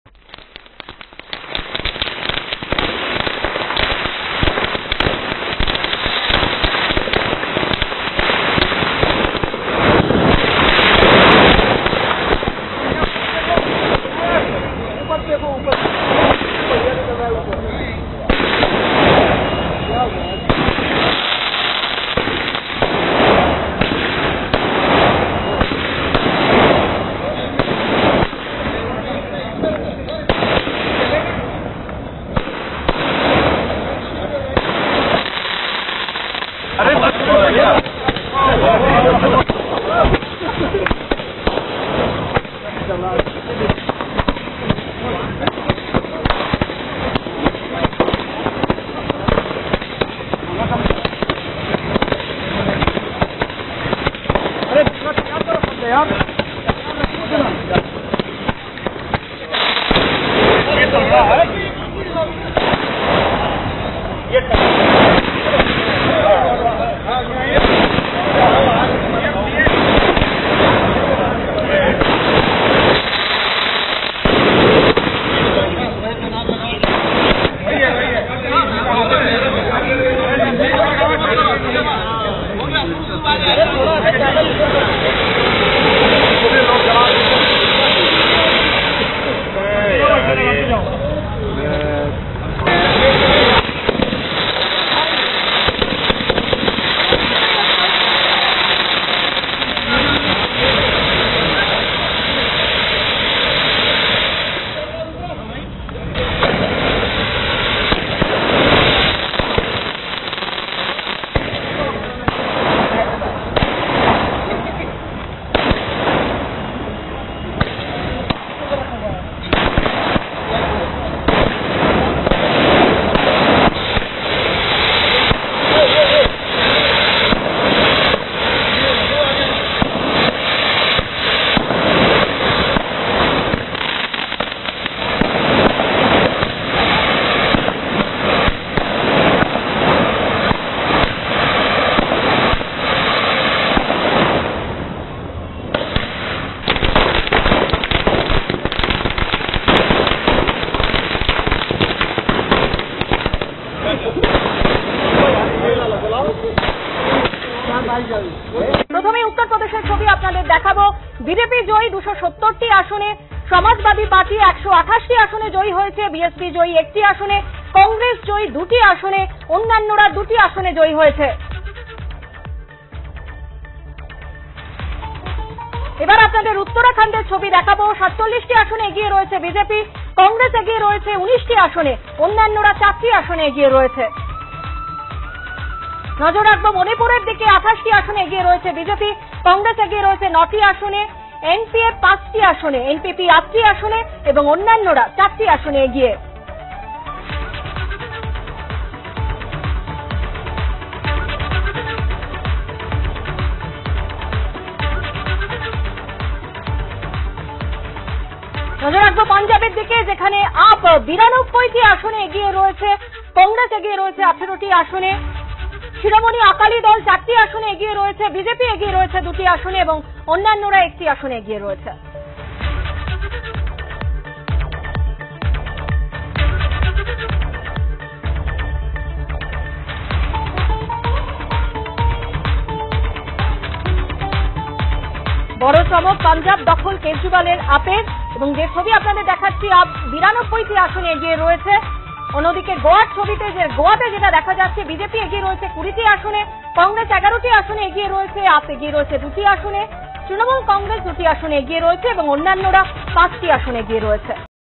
Assembly Election Result 2022: লখনউতে বাজি পুড়িয়ে চলছে জয়ের সেলিব্রেশন । Bangla News
জয়ের আনন্দে মেতে উঠেছে বিজেপি কর্মী ও সমর্থকেরা। লখনউতে সেলিব্রেশনে মেতেছে গেরুয়া শিবির।